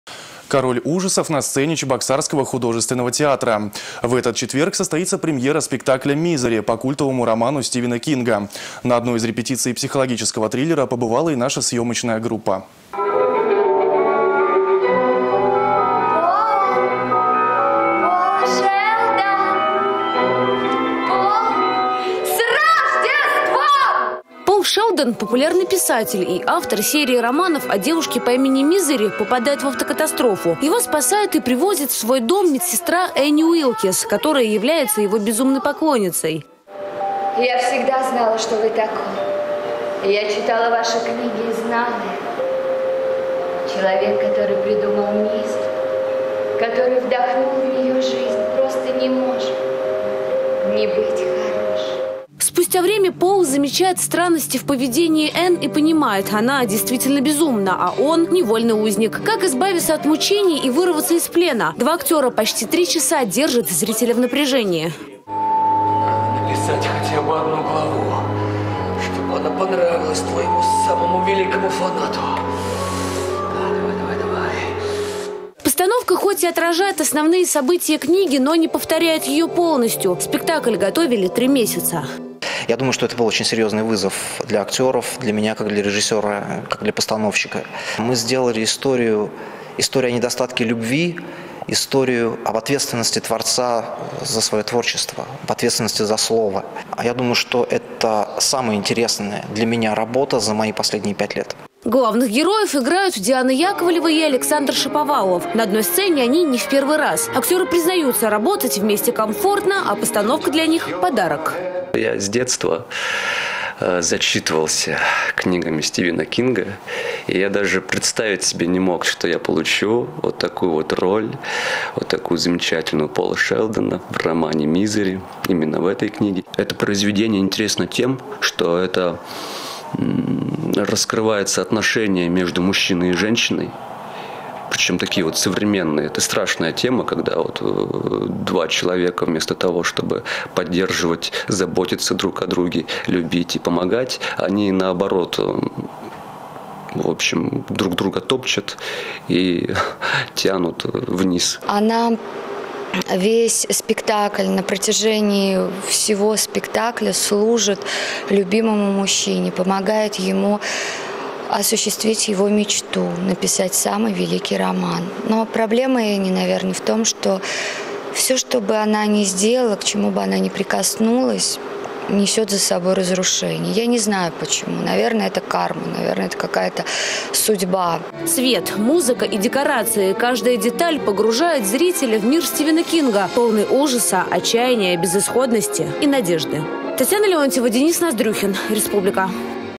«Король ужасов» на сцене чебоксарского художественного театра.
В этот четверг состоится премьера спектакля «Мизери» по культовому роману Стивена Кинга. На одной из репетиций психологического триллера побывала и наша съемочная группа.